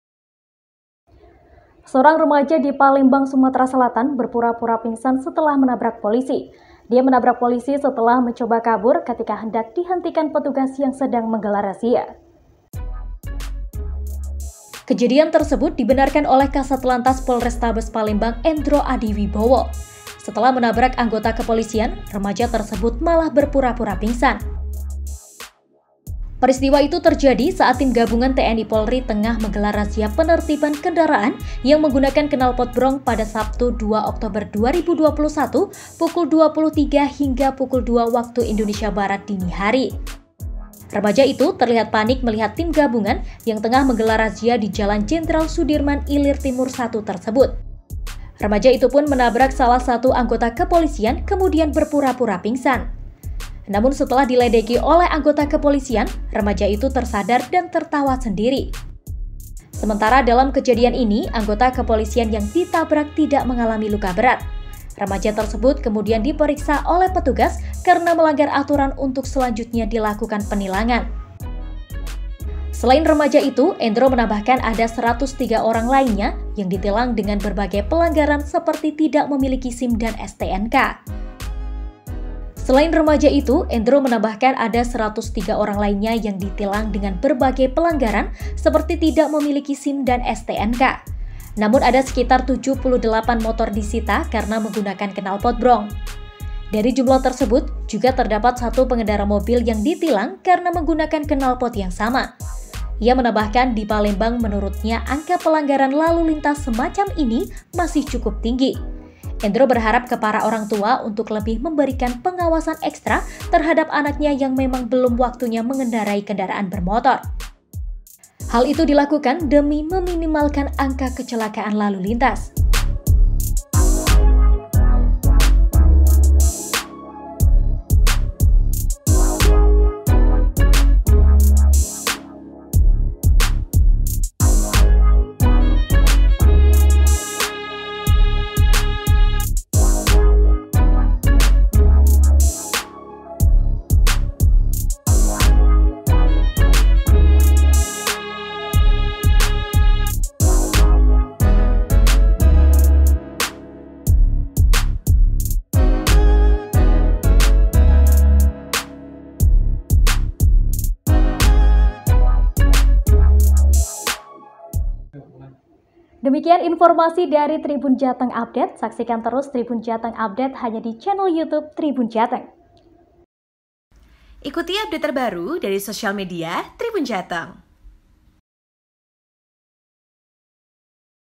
Remaja Pemotor Kaget Ada Razia Tabrak Polisi Pura pura Pingsan
Seorang remaja pria di Palembang, Sumatera Selatan, berpura-pura pingsan setelah menabrak polisi. Dia menabrak polisi setelah mencoba kabur ketika hendak dihentikan petugas yang sedang menggelar razia knalpot brong.

Kejadian tersebut dibenarkan oleh Kasat Lantas Polrestabes Palembang Endro Adi Wibowo. Setelah menabrak anggota kepolisian,  remaja tersebut malah berpura-pura pingsan.